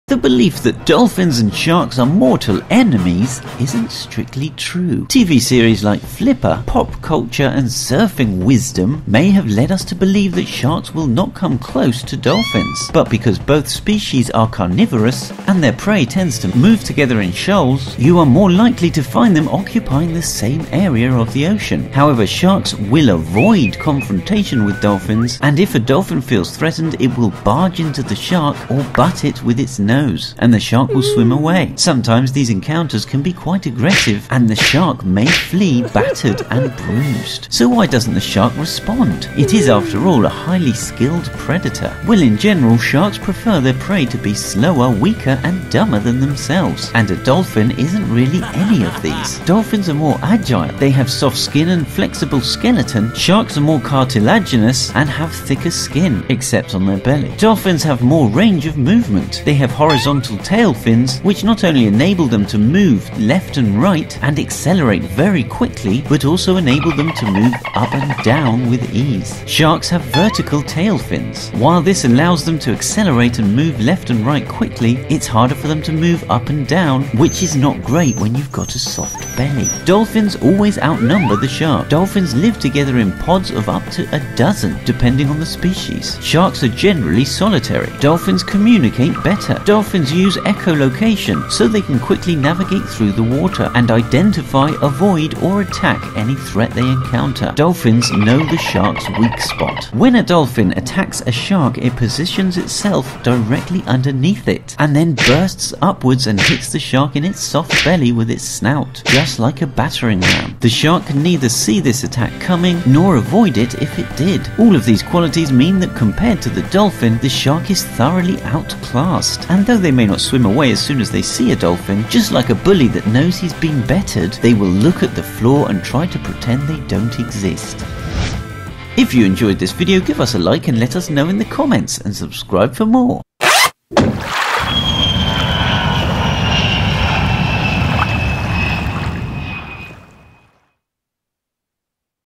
Why Are Sharks Afraid of Dolphins???
The belief that dolphins and sharks are mortal enemies isn't strictly true. TV series like "Flipper", pop culture, and surfing wisdom may have led us to believe that sharks will not come close to dolphins, but because both species are carnivores, and their prey tends to move together in shoals, you are more than likely to find them occupying the same area of ocean. However, sharks will avoid confrontation with dolphins, and if a dolphin feels threatened it will barge into the shark or butt it with its nose and the shark will swim away. Sometimes these encounters can be quite aggressive and the shark may flee battered and bruised.

So why doesn't the shark respond? it is, after all, a highly skilled predator. Well, in general, sharks prefer their prey to be slower, weaker, and dumber than themselves and a Dolphin isn't really any of these,
-Dolphins are more agile.
They have soft skin and a flexible skeleton, sharks are more cartilaginous and have thicker skin, (except on their belly),
-Dolphins have more range of movement.
They have horizontal tail fins this not only enables them to move left and right and accelerate very quickly but also enables them to move up and down with ease.
Sharks have vertical tail fins, while this allows them to accelerate and move left and right quickly, its harder for them to move up and down (which is not great when you've got a soft belly)
-Dolphins always outnumber the shark
Dolphins live together in Pods of up to a dozen, depending on the species. Sharks are generally solitary.
-Dolphins communicate better.
Dolphins use Echolocation, so they can quickly navigate through the water, and identify, avoid, or attack any threat they encounter.
-Dolphins know the shark's weak spot.
When a Dolphin attacks a shark, it positions itself directly underneath it and then bursts upwards and hits the shark in its soft belly with its snout, just like a battering ram.
The shark can neither see this attack coming nor avoid it if it did.
All of these qualities mean that compared to the dolphin, the shark is thoroughly outclassed. and though they may not swim away as soon as they see a dolphin,
Just like a bully that knows he's been bettered, they will look at the floor and try to pretend they don't exist.